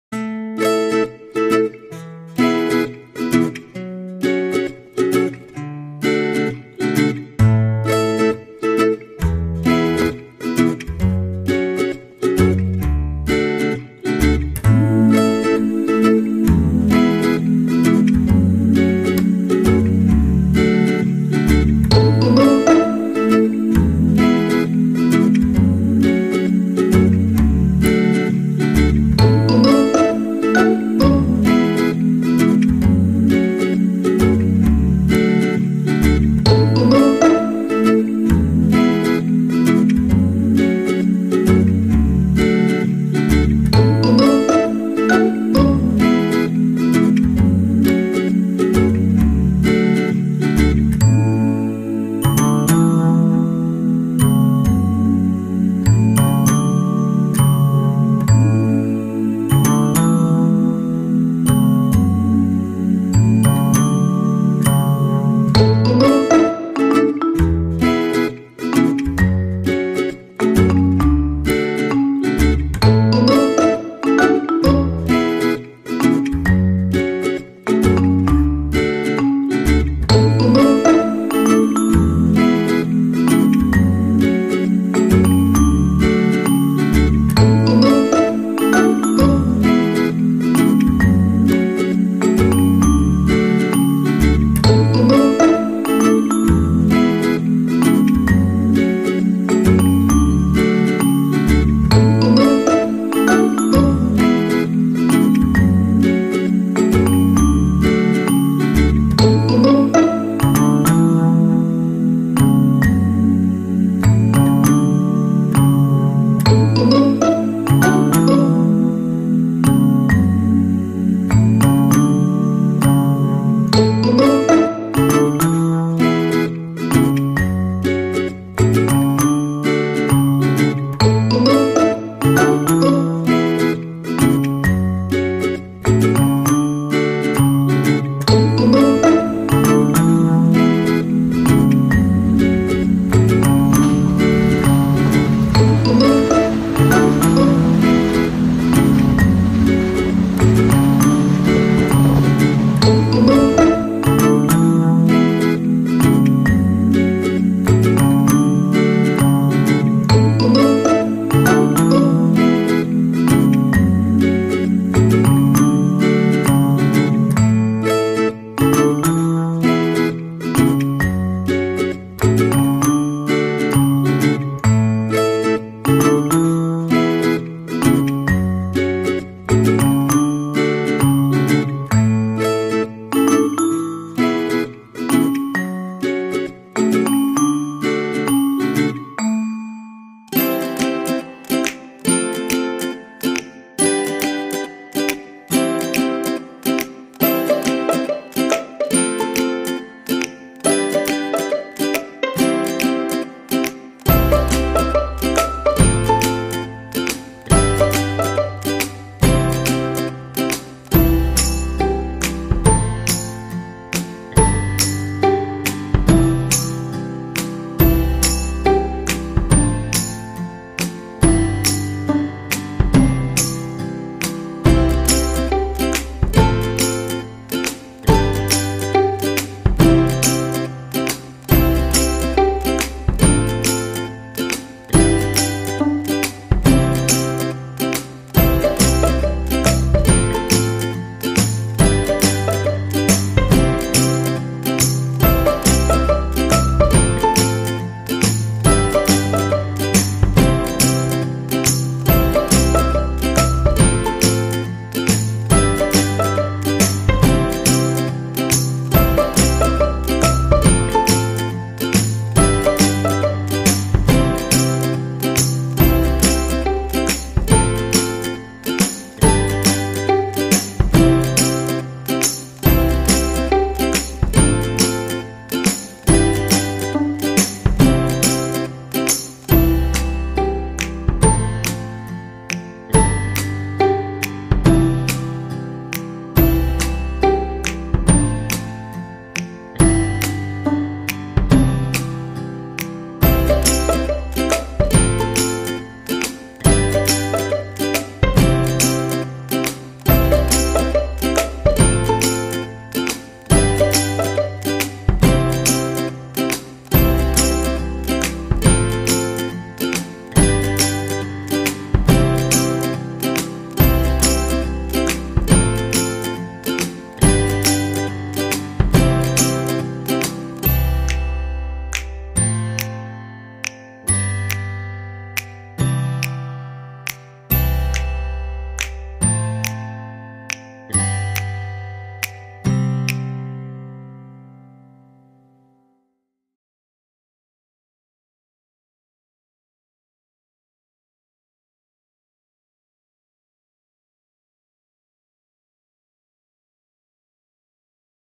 Morning Walk in June 2017
There is a vegetable field near my residence area. I have been taking a walk eveyday for 30 min.  It is ineresting to see how vegetables grow up and are harvested.